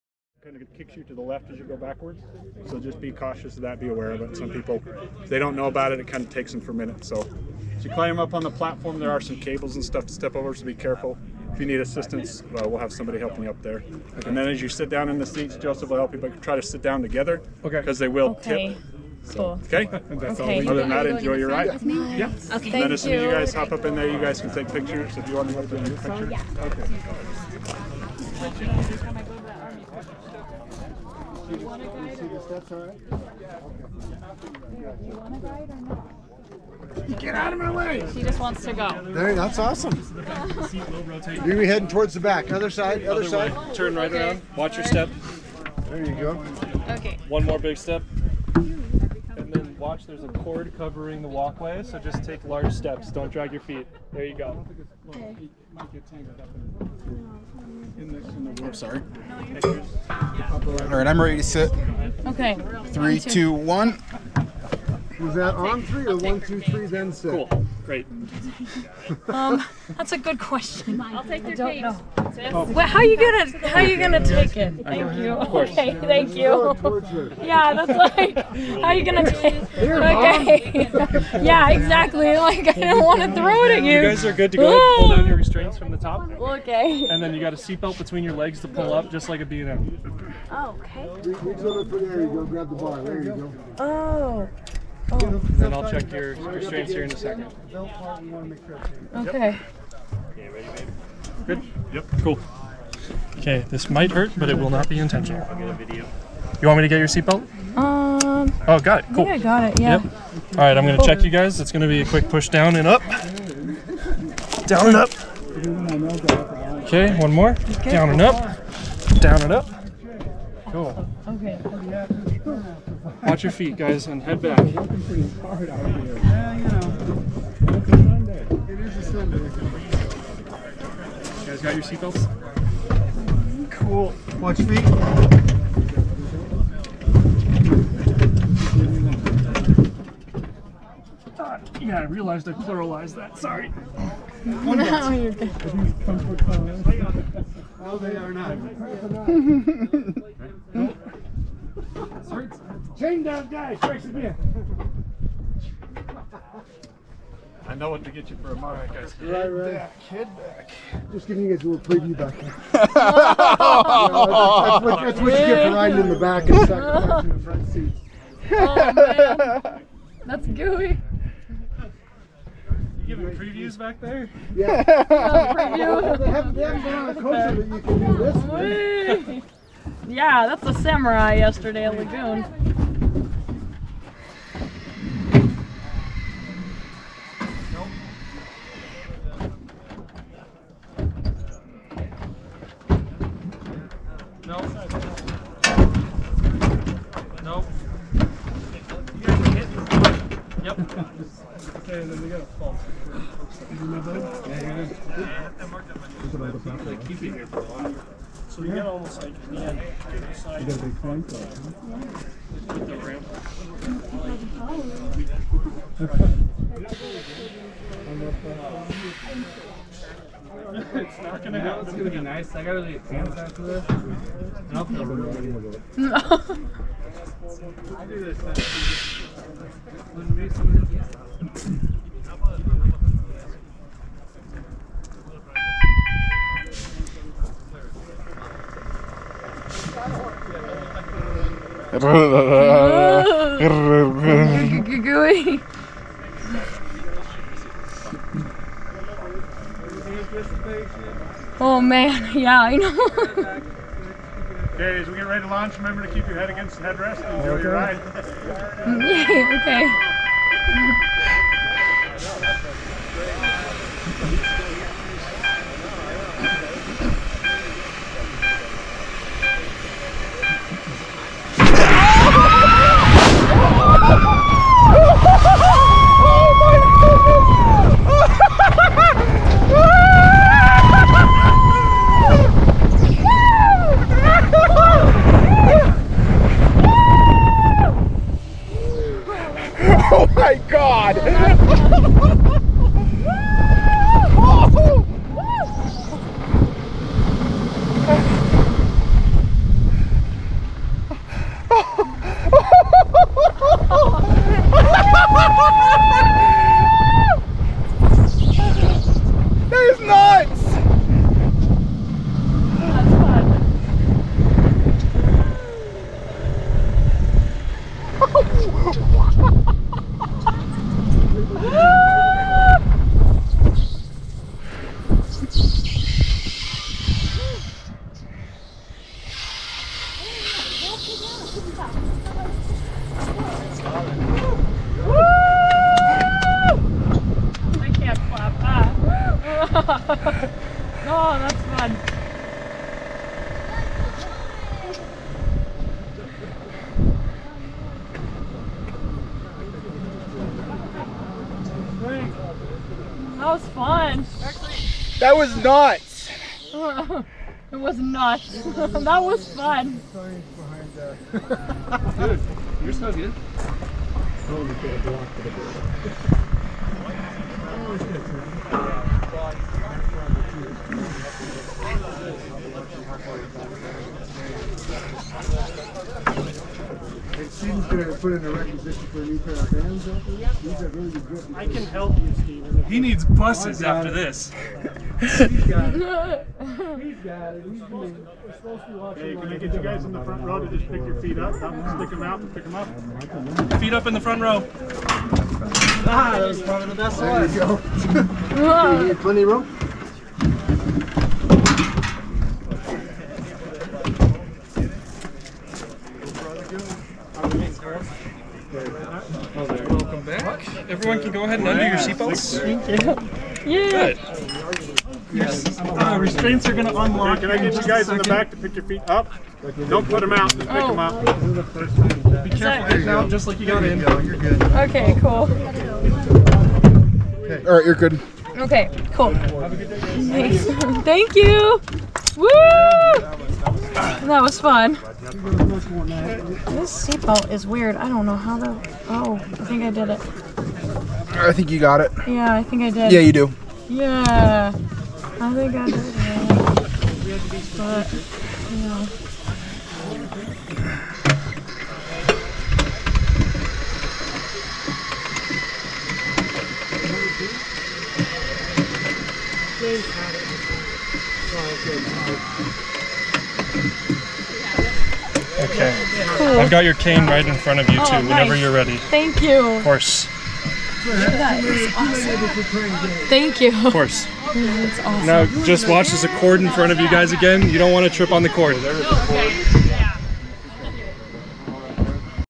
S&S AXIS Prototype POV + Reaction
Shoutouts to @johnychen for allowing me to use his GoPro Max to film this!  Check out his other coaster PoVs at his channel!

Recorded October 1st, 2023 during the ACE Fall Conference.  Thank you S&S for inviting us to ride your prototype!  Camera was used on the ride with S&S's permission.  Always check with the rules of the park you're visiting before recording on any ride.